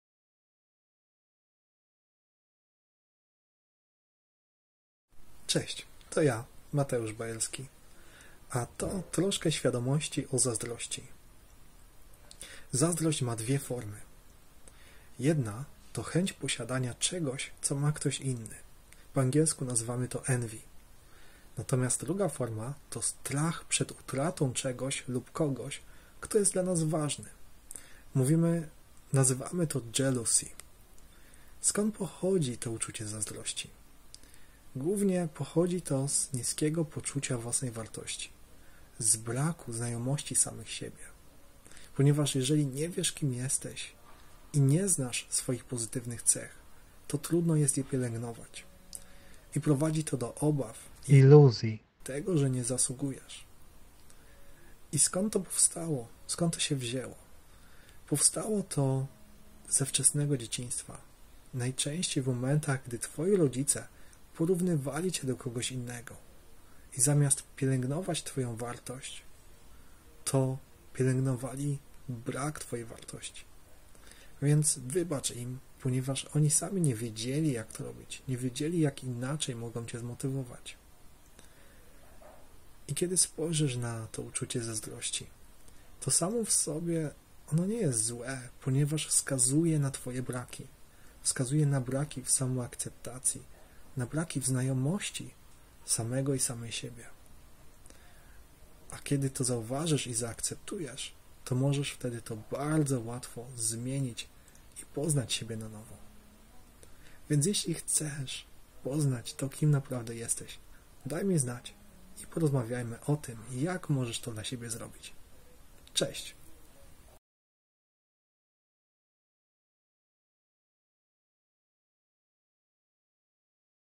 Zazdrość: Jak Niszczy Twoje Zdrowie i Jak Się Od Niej Uwolnić!
Jak zazdrość szkodzi naszemu zdrowiu fizycznemu?

Profesjonalna pomoc:
Jeśli zazdrość staje się przytłaczająca, warto skonsultować się z terapeutą lub hipnoterapeutą, który pomoże nam zrozumieć i przepracować nasze emocje.

Mateusz Bajerski
Accreditations:
✧ DHP Acc. Hyp.
✧ BA Philosophy-Interpersonal Communication, MA Cognitive Science
✧ Member of the Professional Hypnotherapy Practitioner Association